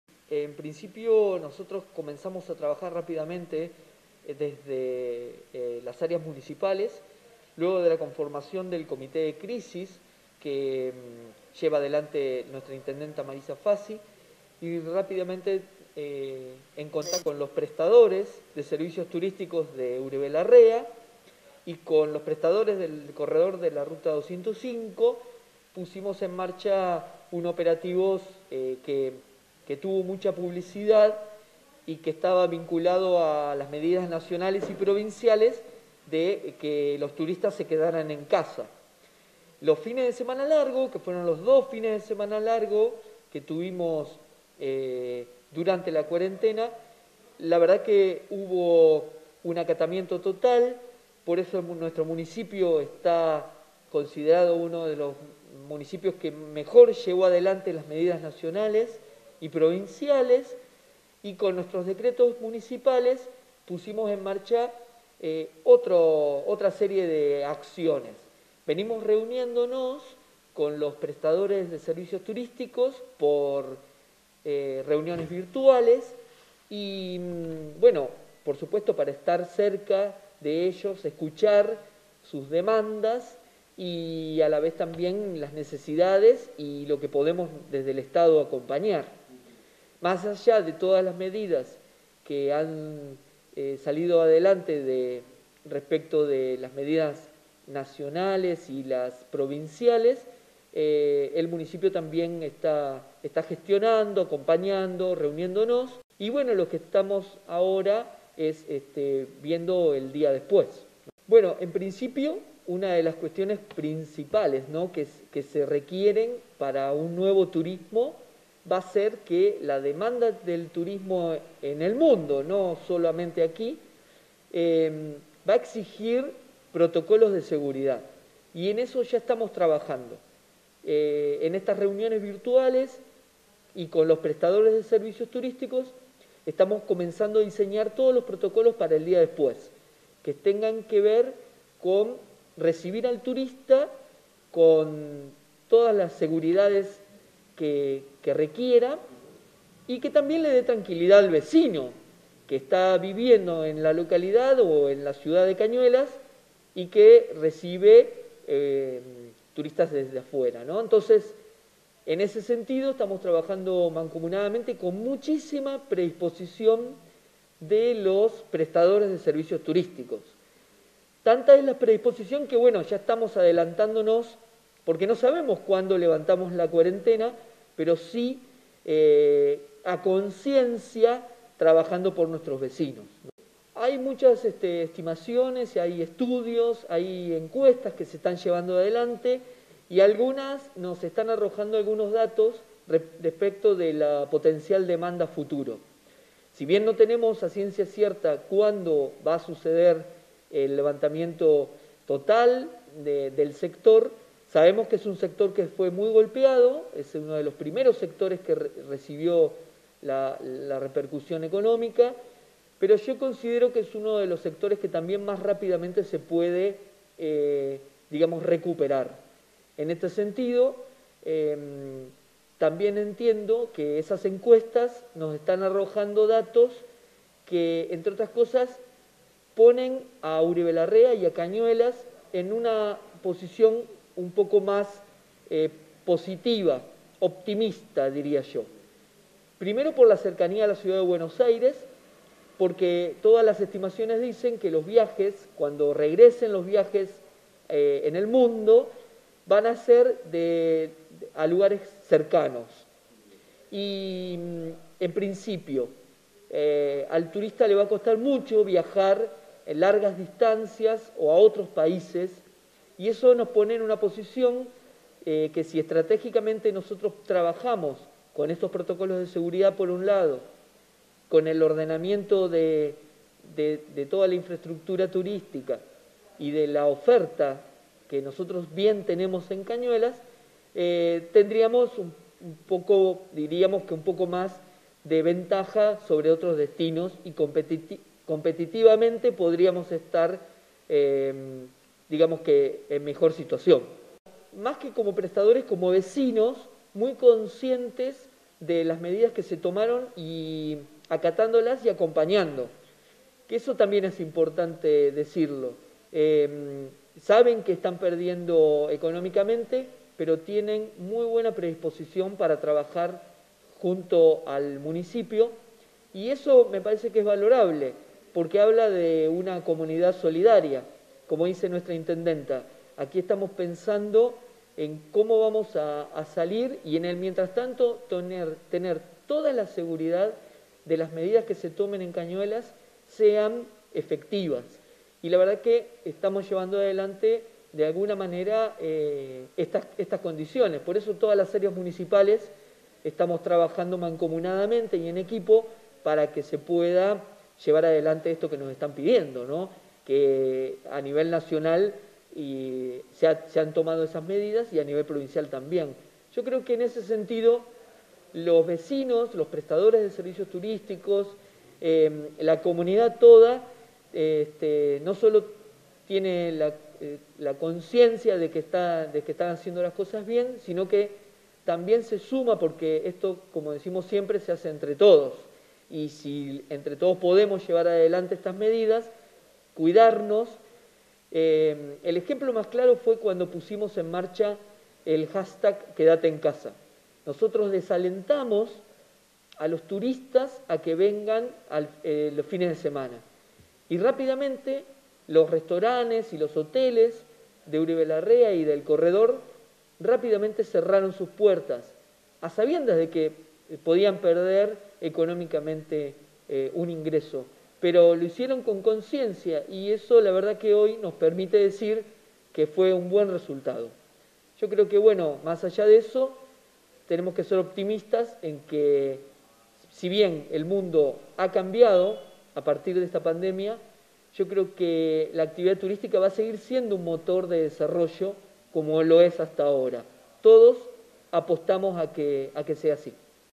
Anticipan que "se viene un nuevo turismo"
Profesionales del sector discuten sobre los protocolos para "el día después" de la cuarentena. Hay expectativas sobre un repunte rápido en la actividad. El subsecretario de Planificación y Desarrollo y licenciado en Turismo, Marcelo Di Giacomo, brindó precisiones a NacPop TV.